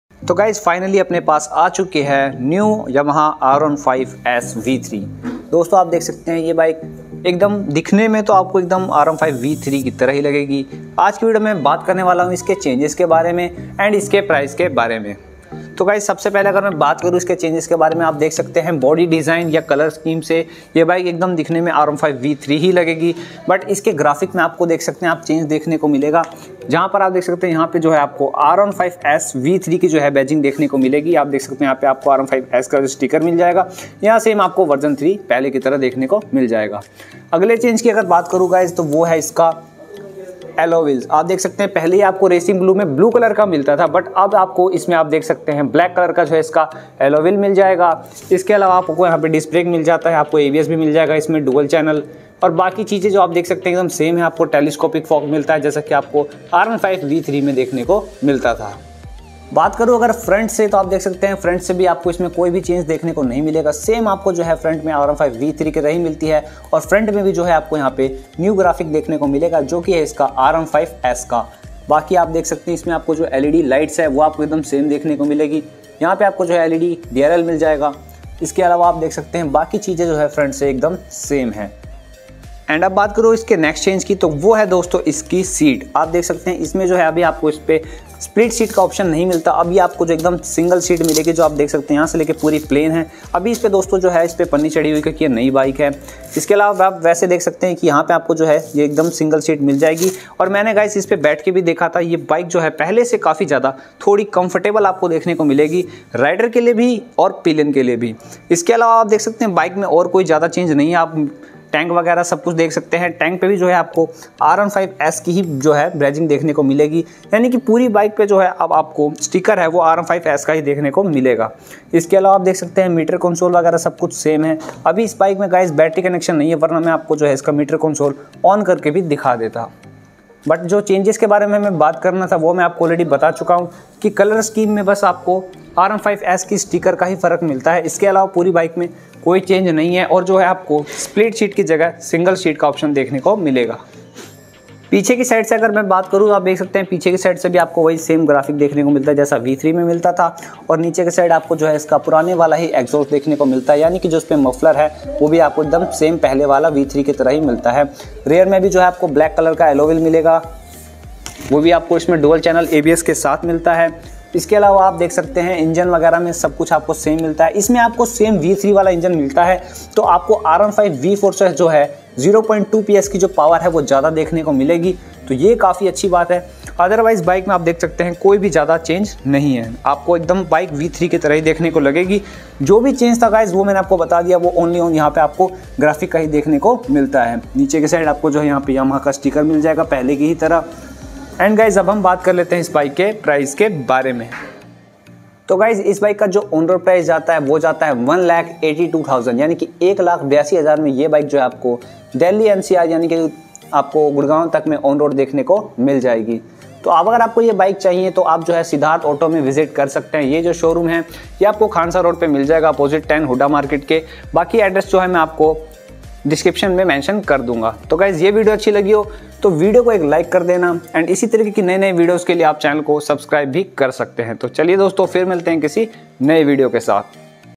2021 New Yamaha R15 S v3 Full Detailed Walkaround Review Change Price 720P HD
Video Credit :- Crazy Motors ( YOUTUBE CHANNEL )

2021 New Yamaha R15 S v3 Full Detailed Walkaround Review   Change   Price  720P HD